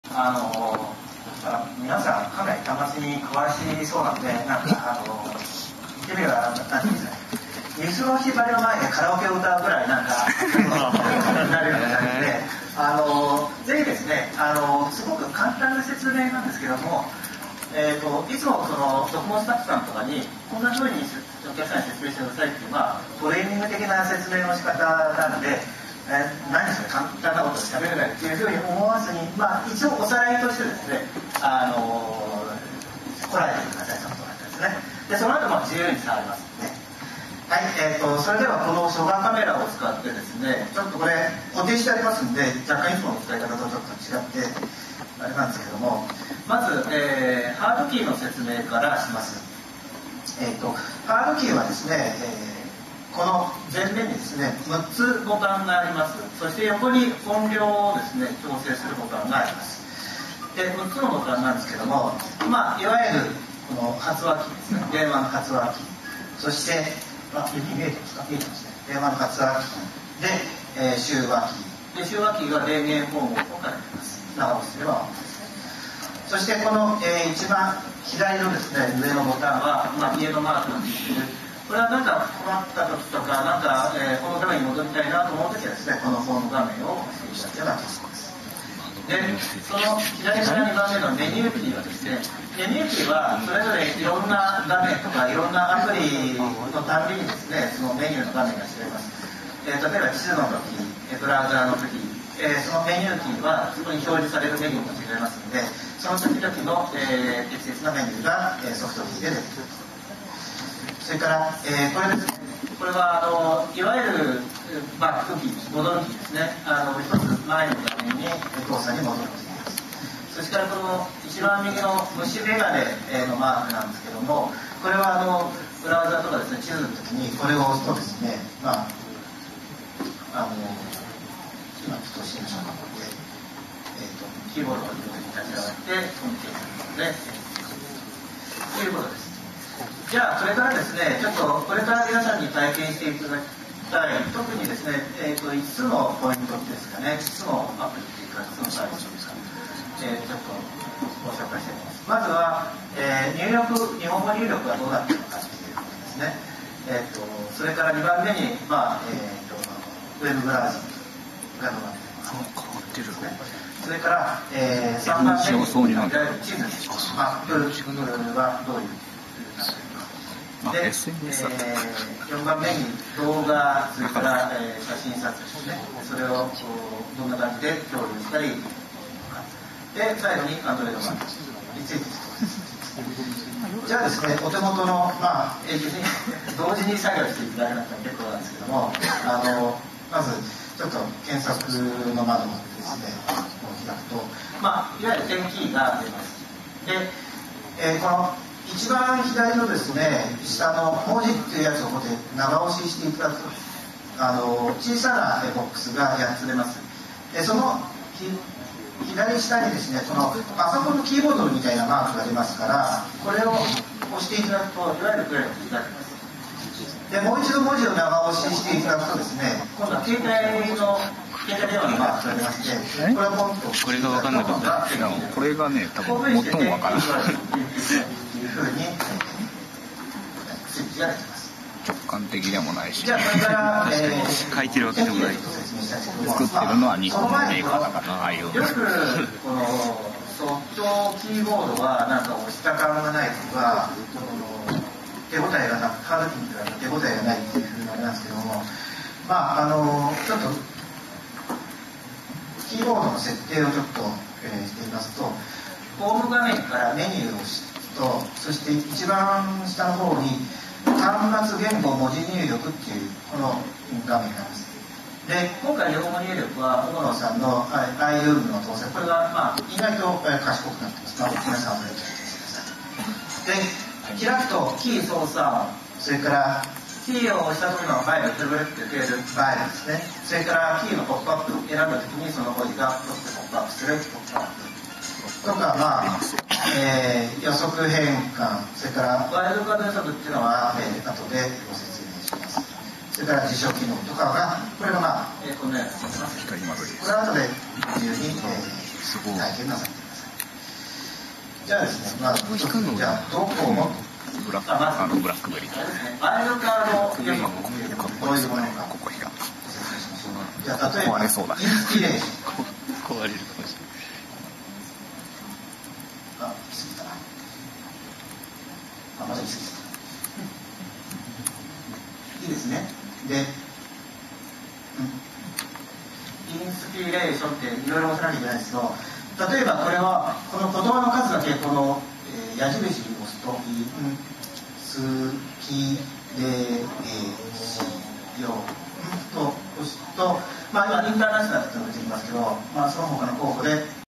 How to operate of "docomo PRO series HT-03A" in the blogger event
2009年7月9日(金)に行われた国内初Androidケータイ「docomo PRO series HT-03A」のブロガー向けイベントにおけるHTC Nippon 田中氏による操作方法を解説した動画です。